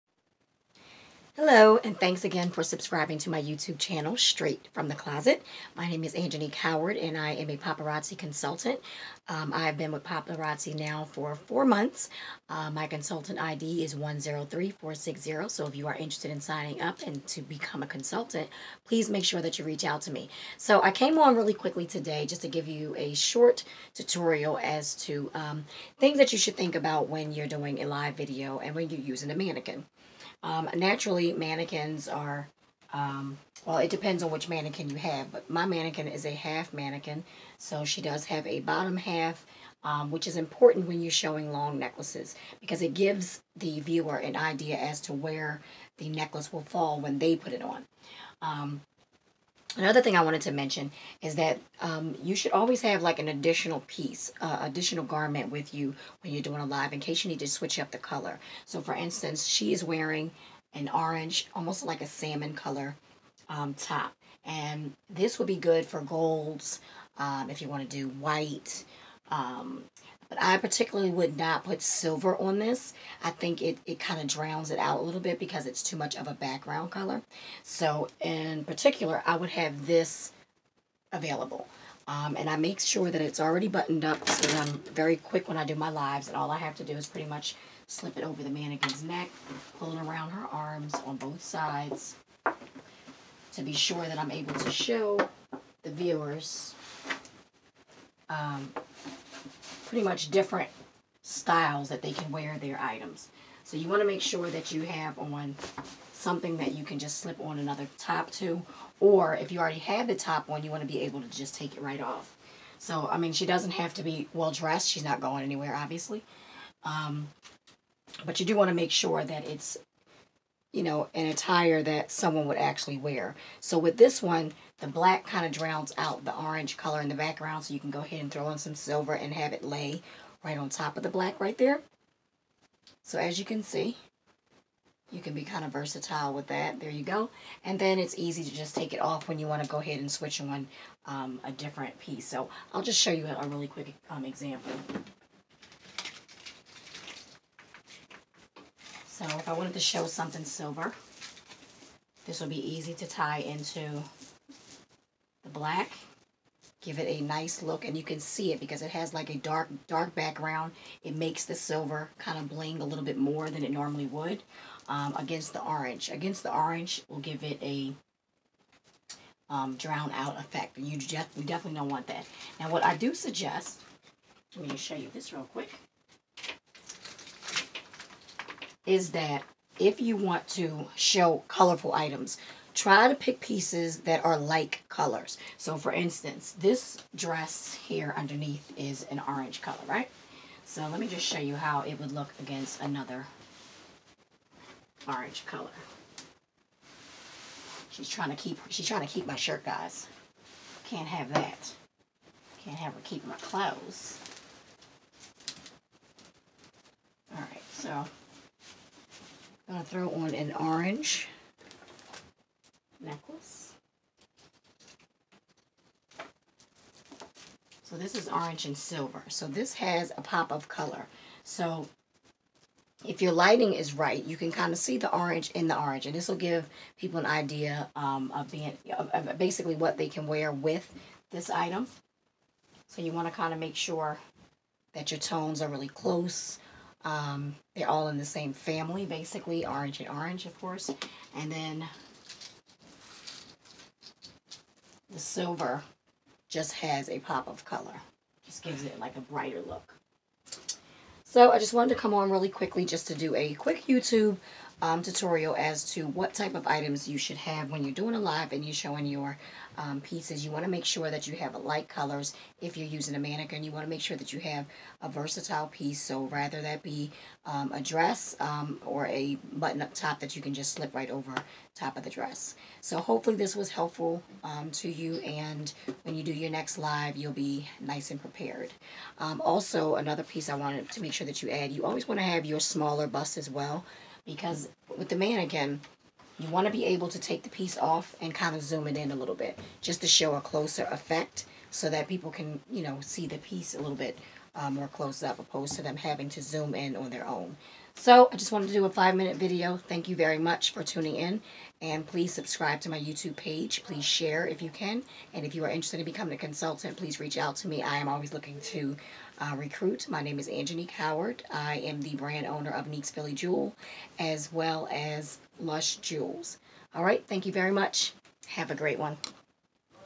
Paparazzi mannequin attire
Quick tips on colors that clash with silver & gold
Lush Jewels by Nique